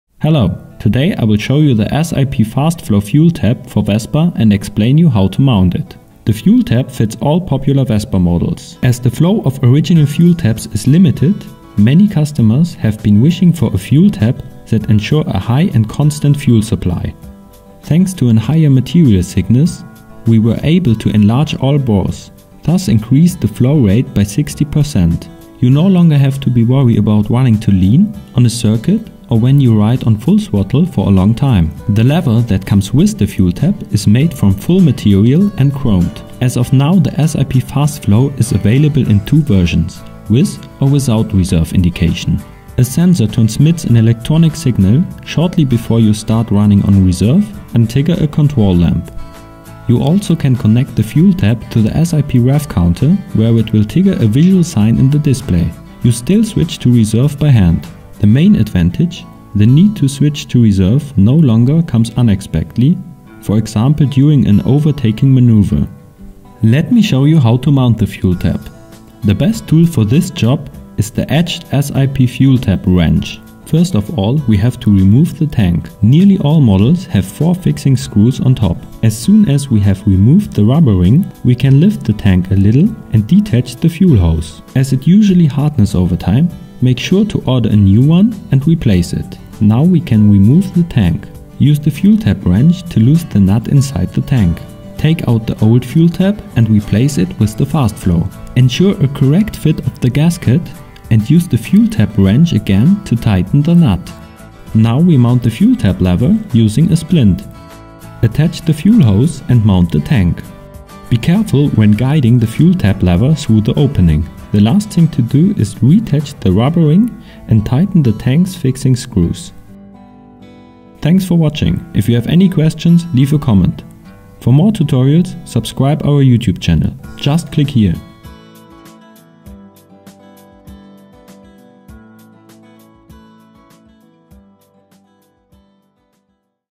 Tutorial: SIP Fastflow fuel tap for Vespa - introducing and mounting
In this video, we show you how to mount the new SIP Fastflow and what it´s all about.
The new SIP PERFORMANCE Fast Flow fuel tap enables the fuel to flow significantly faster (depending on the fuel level up to 60% compared to a standard fuel tap). A brand new casting is the innovative basis for a higher material thickness and a bigger bore/flow Ø. In addition to that, the new cast finally allows of a supply tube with a significantly higher diameter. The enlarged tap bit ensures the untroubled flow of the fuel regardless of the lever's position. To sum it up: We did not only increase the bore, but also the effective flow. You find tight rubber sealings around the apertures. An additional sliding disc guarantees simple and easy handling. The fuel tap comes with a chromed solid metal lever. Owing to its perfect manufacture, the new fuel tap is a welcome premium spare part for daily riders, too.

Conclusion: Vespa missed out on this part for years. Now it's finally here. No matter if you drive in normal or reserve mode... the SIP SCOOTERSHOP Fast Flow fuel tap ensures a significantly faster fuel flow than the original fuel tap.

Bore Ø 5,0mm, enlarged tap and handling bit, supply tube inner Ø 5,0mm, increase in fuel flow of up to 60%, smooth-running, incl. sealings and chromed metal fuel lever.

If you like it, tell everybody and spread the video.